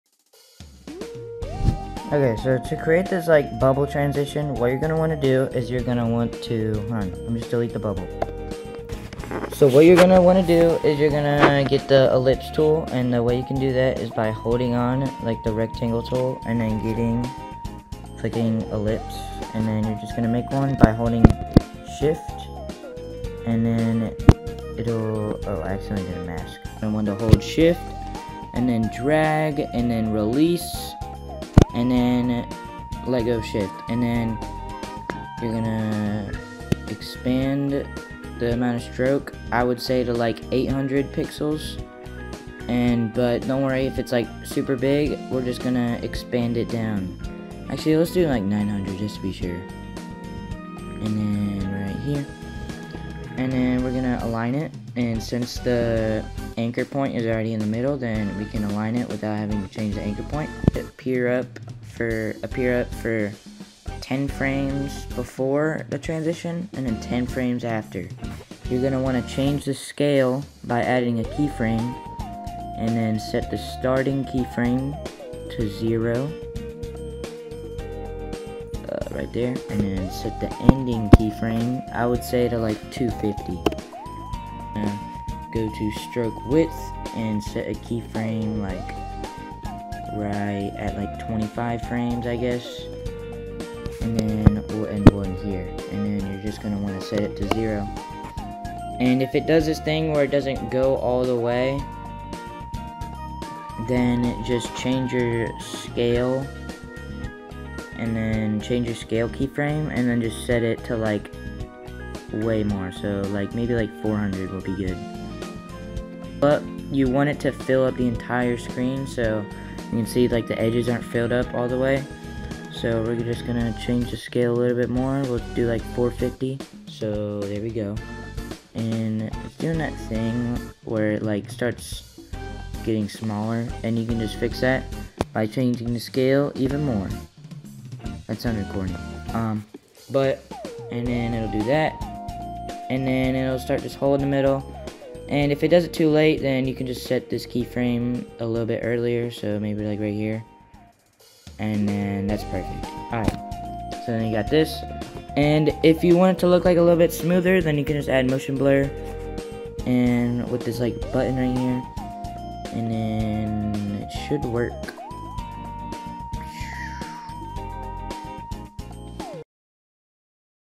How to Make an EASY TRANSITION After Effects Tutorial
This is a simple but easy transition to use in edits, videos, and other content.
It is great for beginners and is a good learning opportunity for those who want to learn transitions and how they work.